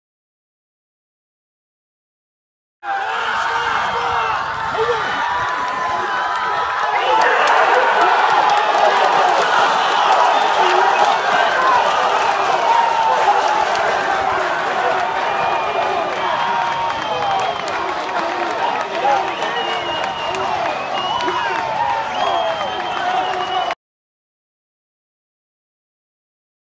WATCH Final Play At Plate Of Tottenville's PSAL Quarterfinal Win
Sanacore's throw to Wise catches Romolo for final out of 4-3 win